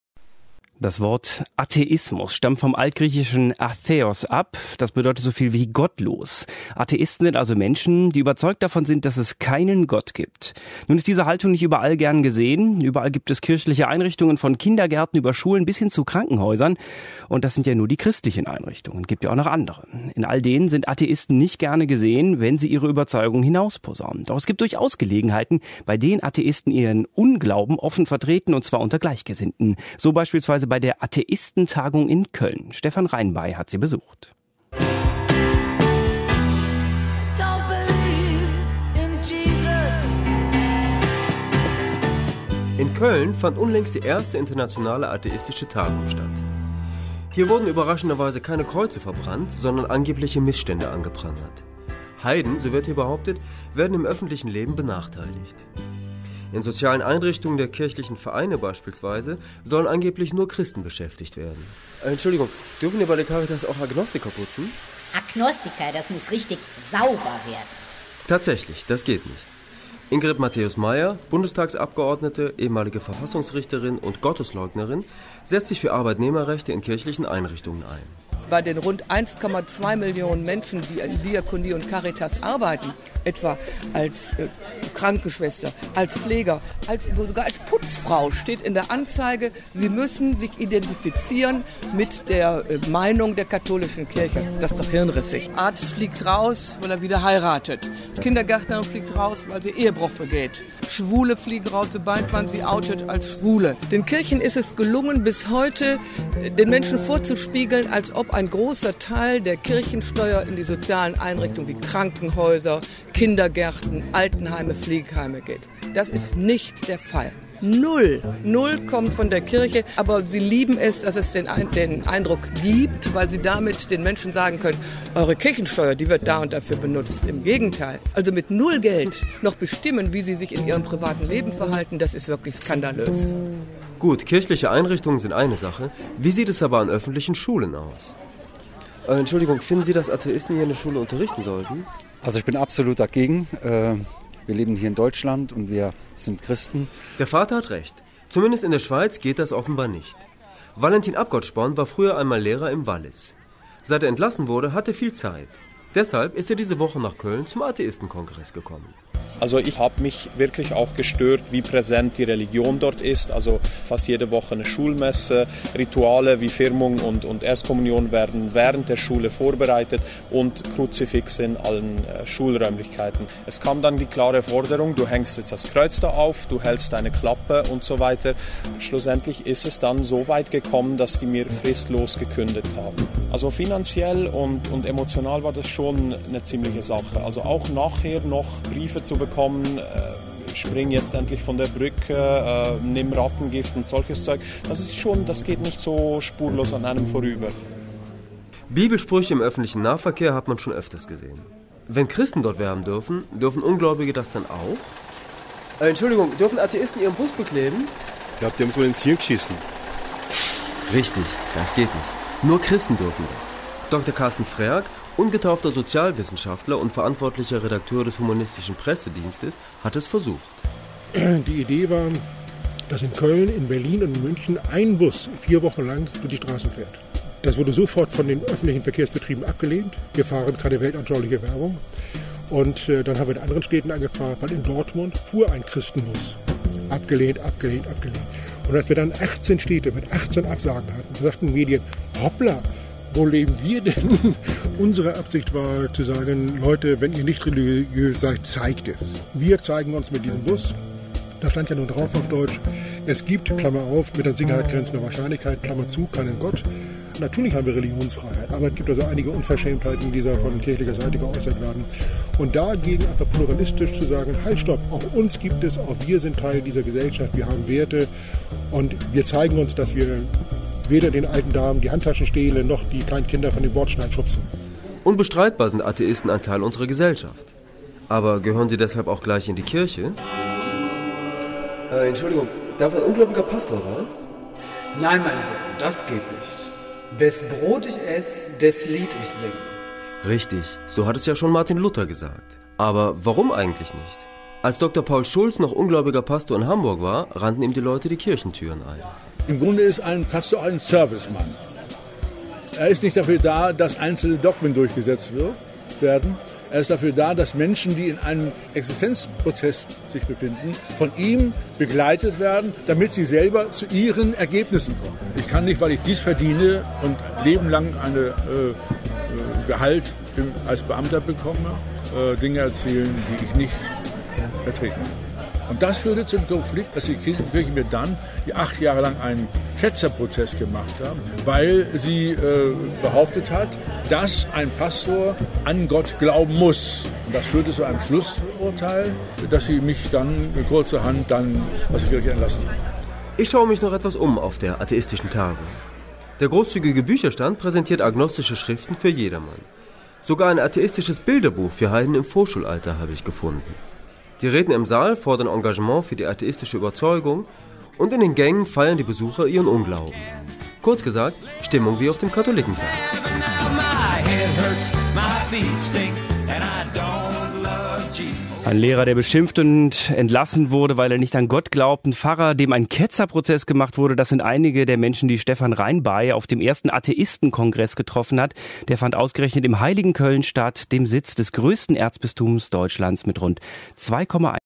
Atheismus-Tagung : Was wollen Atheisten heute noch erreichen?
Was wollen Atheisten heute noch erreichen? Unser Reporter hat sie gefragt. Auf der "internationalen atheistischen Tagung" in Köln hat er einige Antworten bekommen, unter anderem von der Bundestagsabgeordneten Ingrid Matthäus-Maier und einem atheistischen Pastor.

Wir leben in einer säkularen Gesellschaft. Muss man da als Atheist noch für oder gegen irgendetwas kämpfen? Oder rennen die Atheisten offene Türen ein? Die Redner auf der atheistischen Tagung sehen durchaus noch Handlungsbedarf. Denn sie fühlen sich im öffentlichen Leben benachteiligt.

Ausgerechnet Pfingsten

Während die Christen weltweit das Kommen des Heiligen Geistes feierten, trafen sich die Ungläubigen in Köln. "Es geht aufwärts mit dem Atheismus" schreiben die Veranstalter auf ihrer Homepage. "Das Jahr 2012 wird mit der Zahl atheistischer und humanistischer Konferenzen einen weltweiten Rekord aufstellen." DRadio Wissen wollte wissen, wer die Tagung besucht und wofür ein engagierter Atheist heute steht.

Bild : dpa | picture alliance | Andy Rain